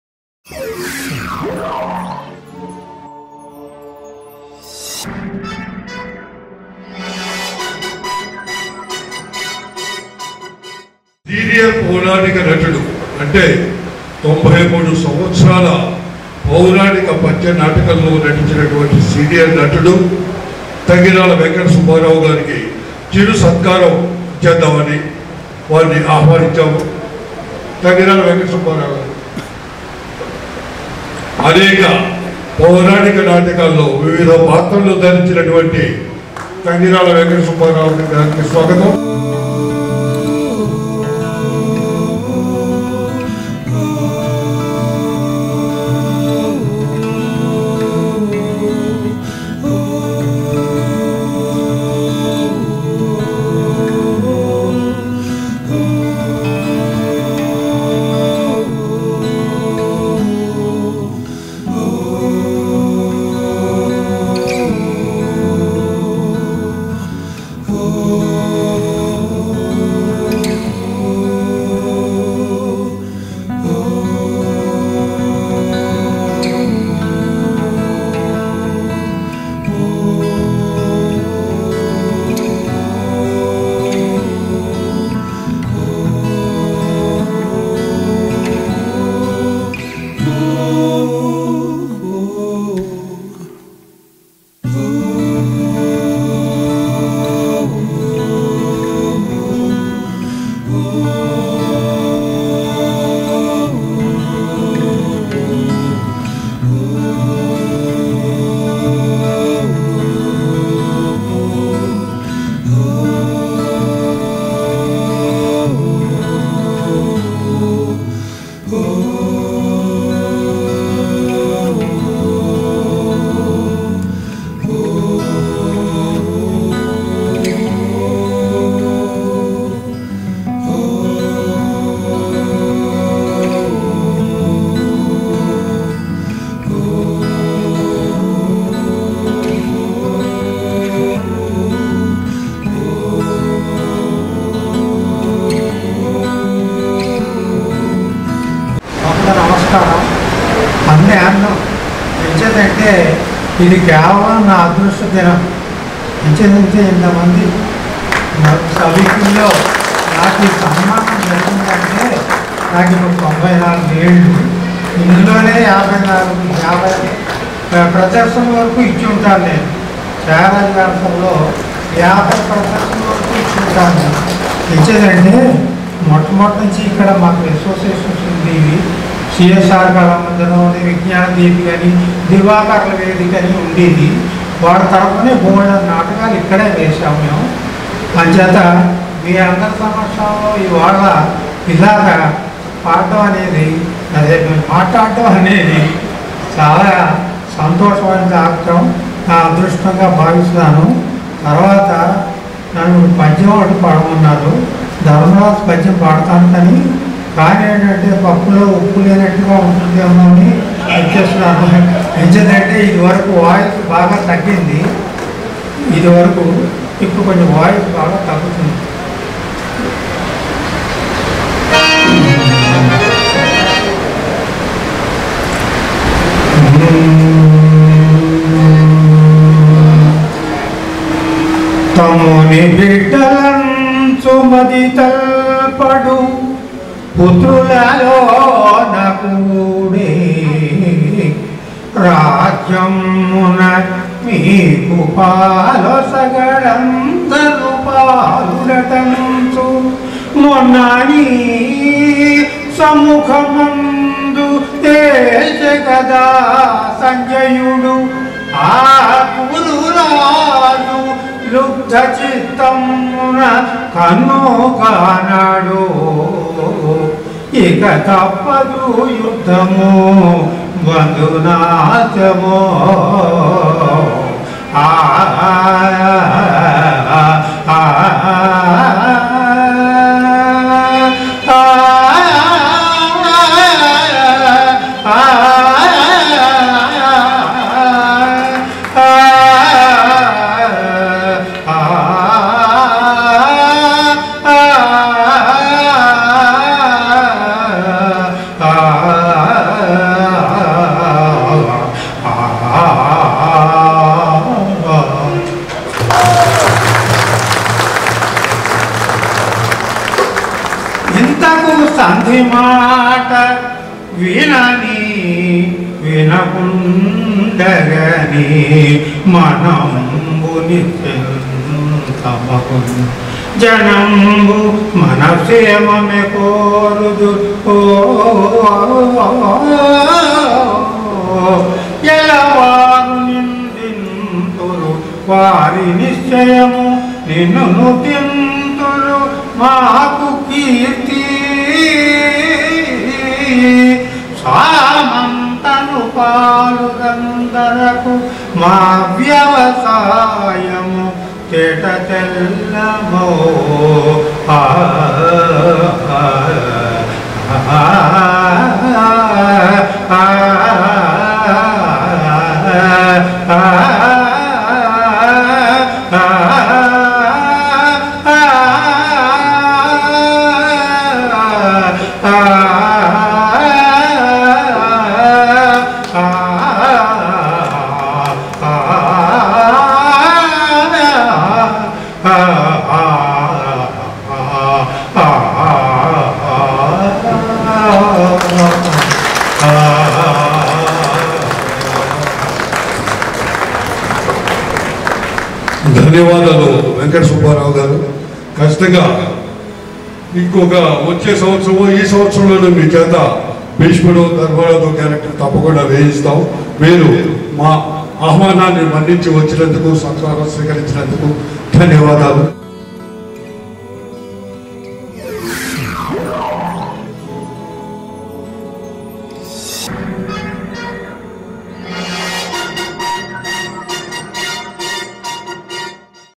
93 ఏళ్ల పౌరాణిక సీనియర్ నటులు వెంకట సుబ్బారావు గారికి ఘనసన్మానం
DOP & Editing : Vidya Sagar